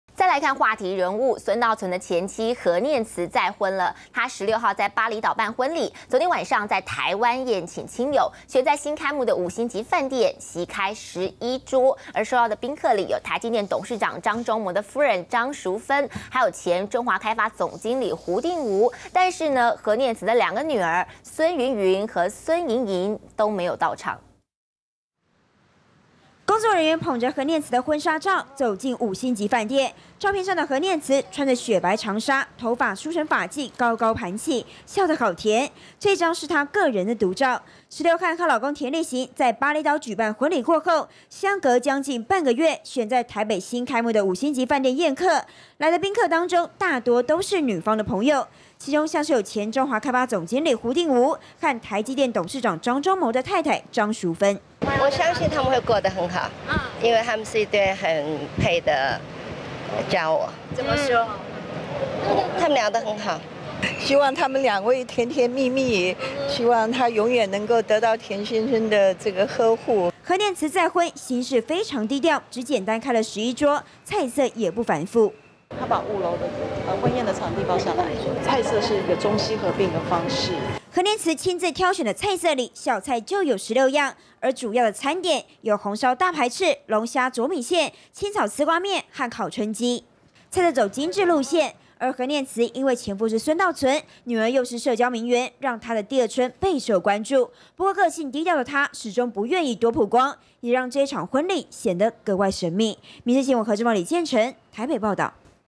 席開11桌 何念慈補請婚宴－民視新聞
【民視即時新聞】孫道存的前妻、名媛孫芸芸的母親何念慈再婚，16號在峇里島舉辦婚禮後，今天晚上在台灣宴請親友，選在新開幕的五星級飯店，席開11桌，受邀的賓客裡，有台積電董事長張忠謀的太太張淑芬，還有前中華開發總經理胡定吾，而何念慈的兩個女兒，孫芸芸和孫瑩瑩都沒有到場。